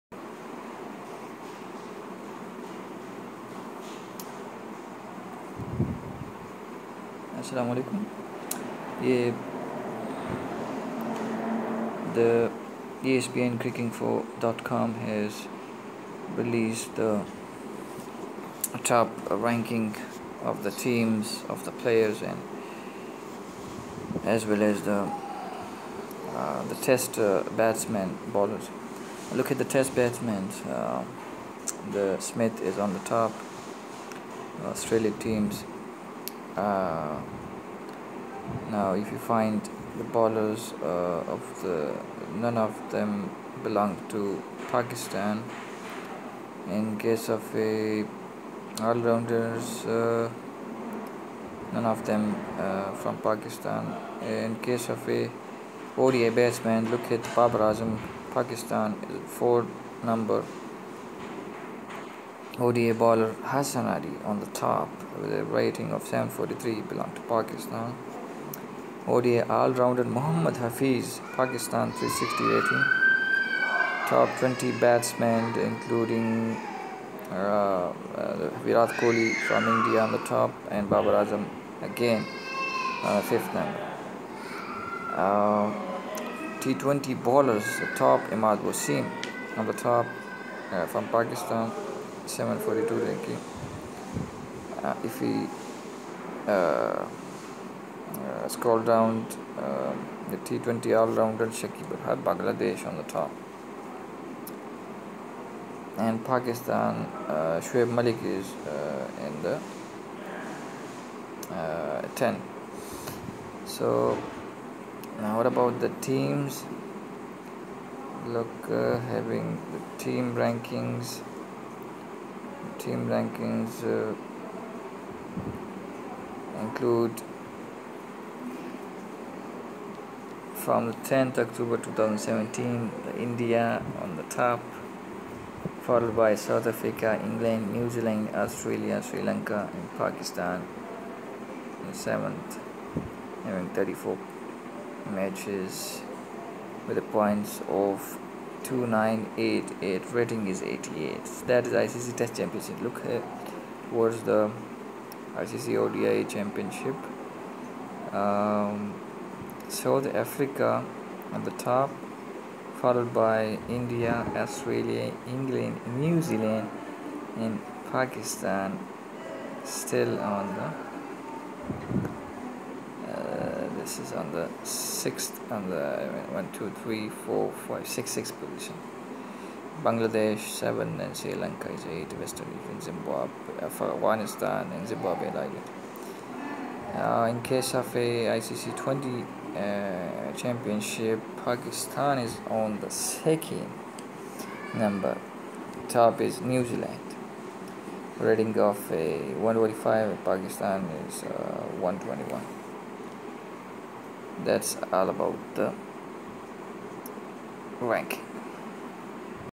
Icc top ranking oct 2017
This video is about the ranking of odi team test teams bowlers and batsman.